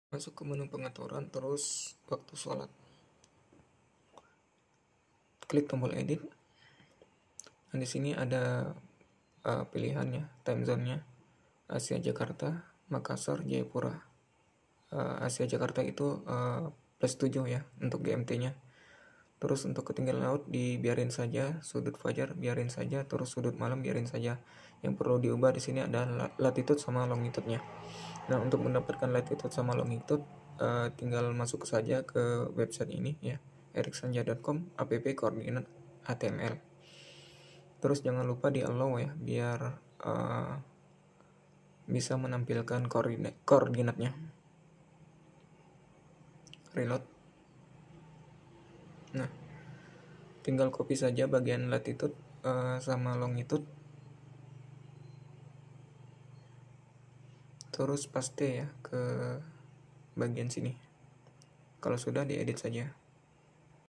jam masjid - 09 - waktu sholat
Aplikasi jam masjid digital.

Hanya Ratusan Ribu, sudah punya aplikasi jam masjid digital 😍
Aplikasi jam masjid digital ini berbasis web, bisa diinstall di komputer / laptop, mini pc & bisa juga dionlinekan.